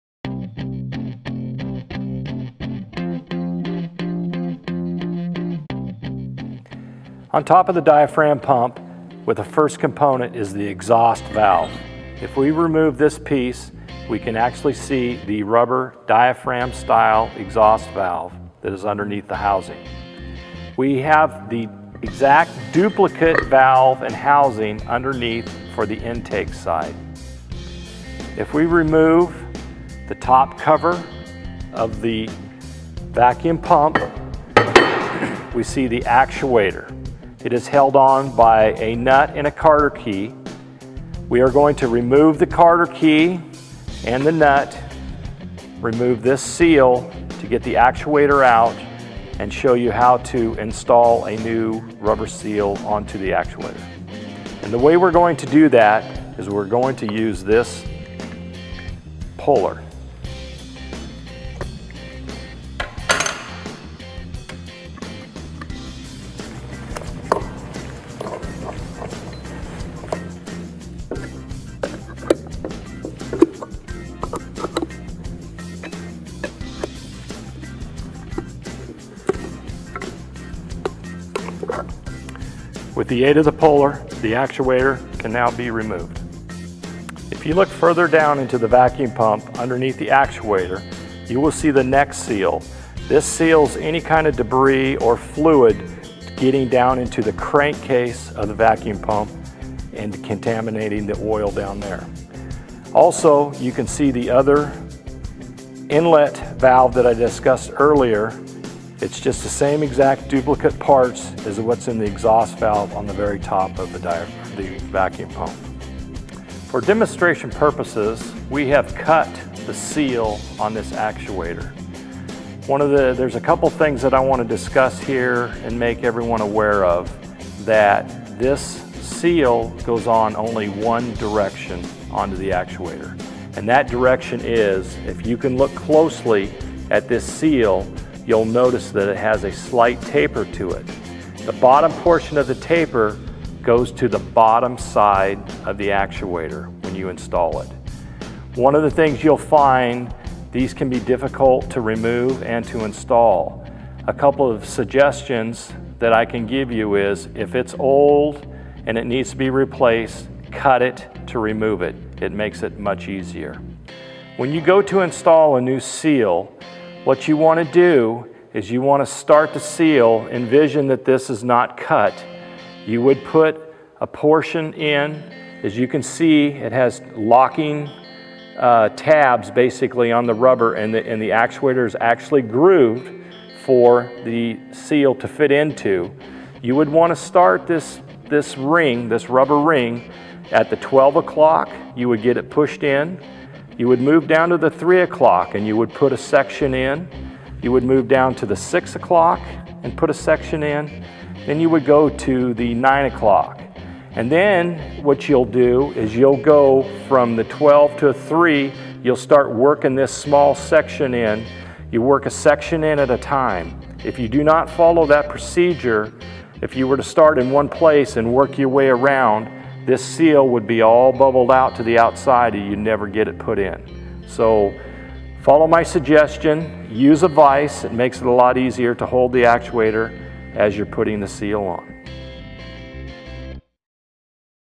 Pioneer Pump Service Training - The Diaphragm Pump
The Diaphragm Pump

This series of videos will illustrate how to disassemble a Pioneer Pump PP88S12. The PP88S12 is an 8" vacuum assisted centrifugal pump capable of handling a 3" solid that is used in applications such as general dewatering and sewage bypass.
In this segment, we are going to disassemble and discuss the vacuum pump.